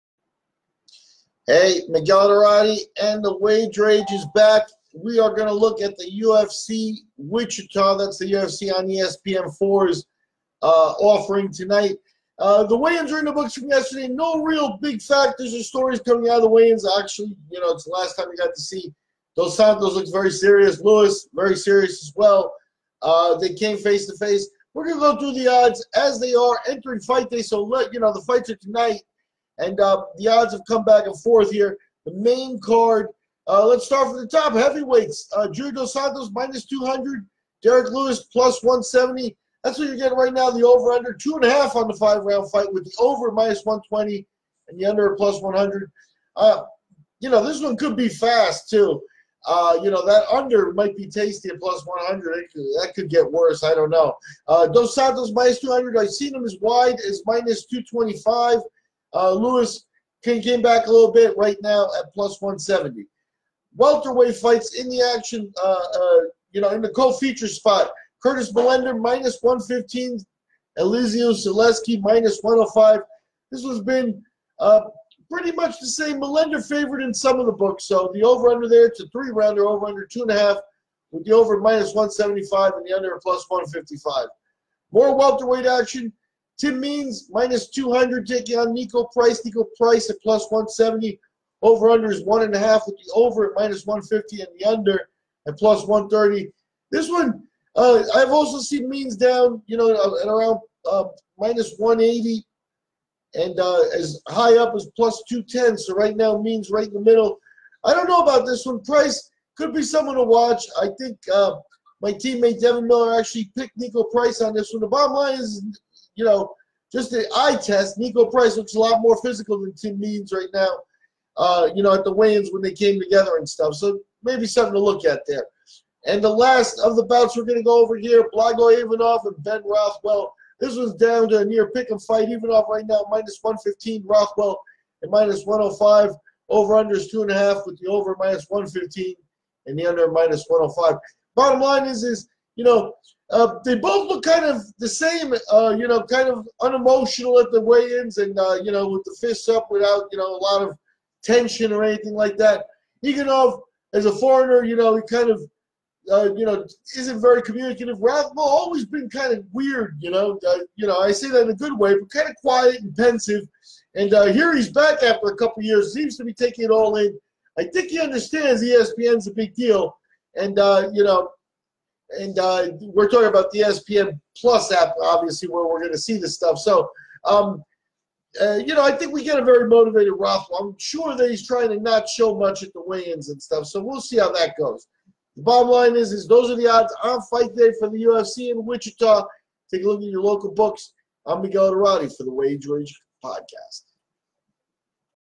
Wage Rage - Last Look at UFC Wichita Odds
It is fight day in Wichita and this latest CRP Wage Rage reviews the odds at the sports books for tonights main fights.

Former heavyweight champion Junior Dos Santos battles contender Derrick Lewis in the heavyweight main event that has seen Dos Santos a roughly 2 to 1 favorite consistently. Now that we have seen Lewis and Dos Santos face to face at the weigh-ins, will money come in on Lewis?

UFC on ESPN+ 4 - INTRUST Bank Arena - Wichita, Kansas - ESPN+ - Saturday, March 9, 2019

Heavyweight 3 rounds -
Ben Rothwell -105 Over 2½ -115
Blagoy Ivanov -115 Under 2½ -105

Welterweight 3 rounds -
Niko Price +170 Over 1½ -150
Tim Means -200 Under 1½ +130

Welterweight 3 rounds -
Elizeu Zaleski -105 Over 2½ -175
Curtis Millender -115 U 2½ +155

Heavyweight 5 rounds -
Derrick Lewis +170 Over 2½ -120
Junior dos Santos -200 U 2½ +100

Take a look at earlier odds and our MMA team's picks for Dos Santos vs Lewis right here: